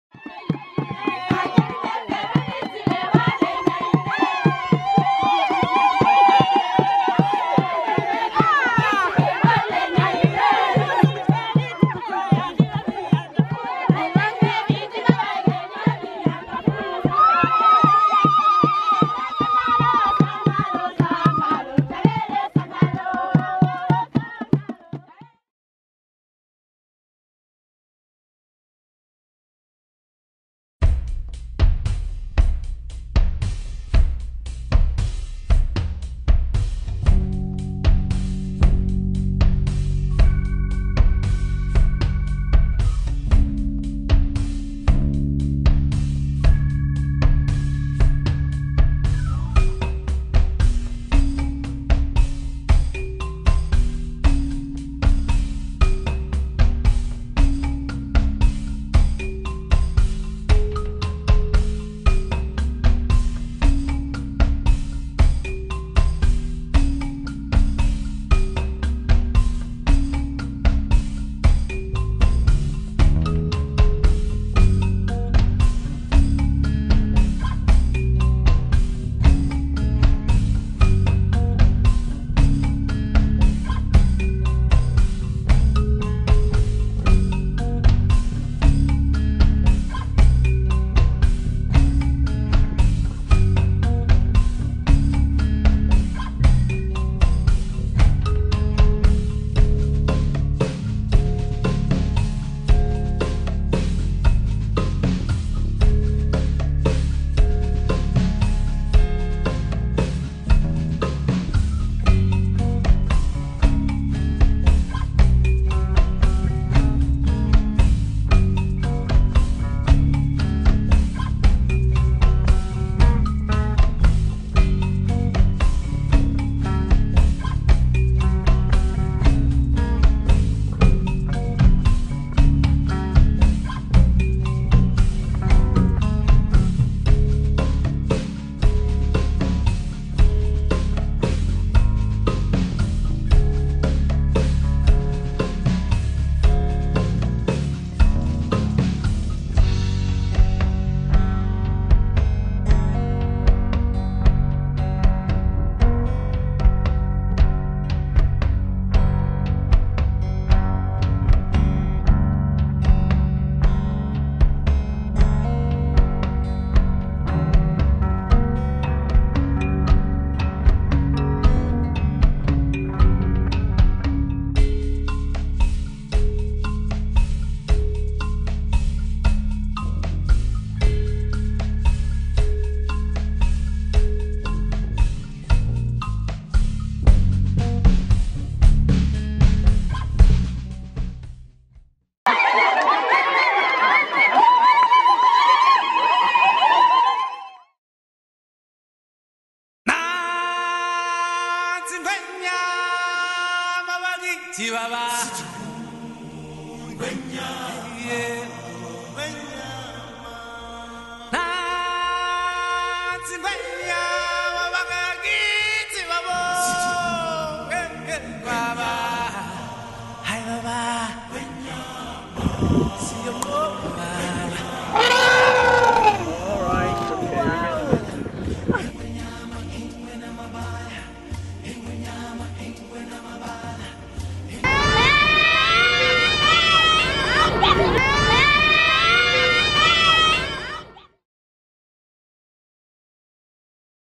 Geography Degree | Volunteering in Uganda
Earlier this year (2017), a group of Geography degree students volunteered at Esiteri Nursery in Bugema, three miles south of Mbale in eastern Uganda.